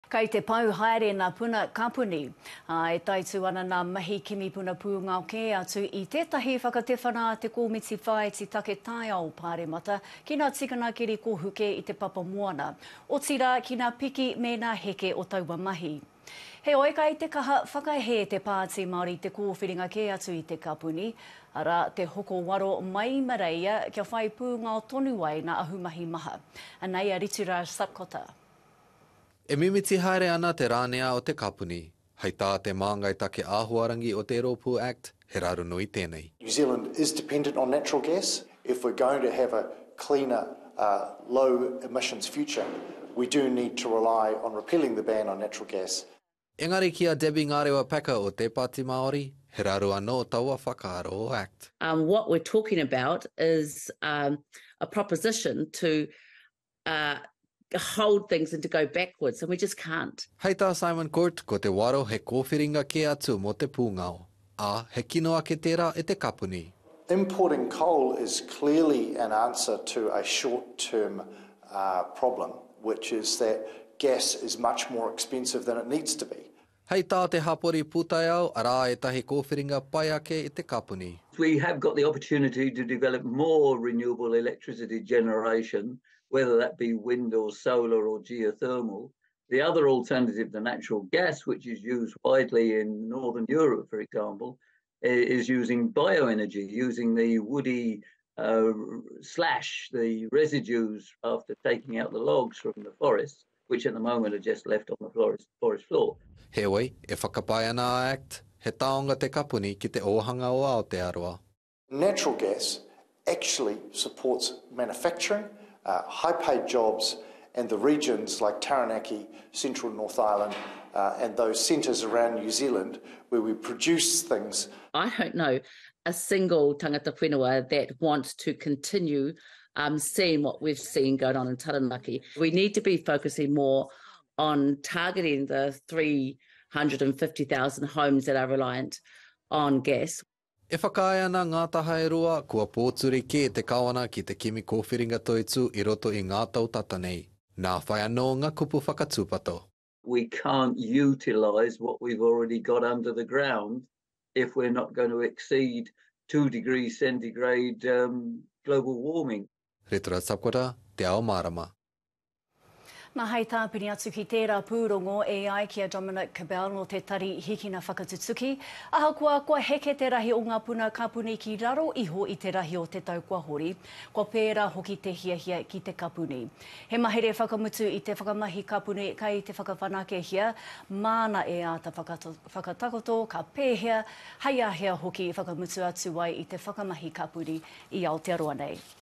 Aotearoa’s natural gas supply is running out: What will replace it?
Natural gas reserves in Aotearoa are rapidly depleting, with an MBIE report released last week showing reserves have dropped below 10 years of remaining stocks for the first time.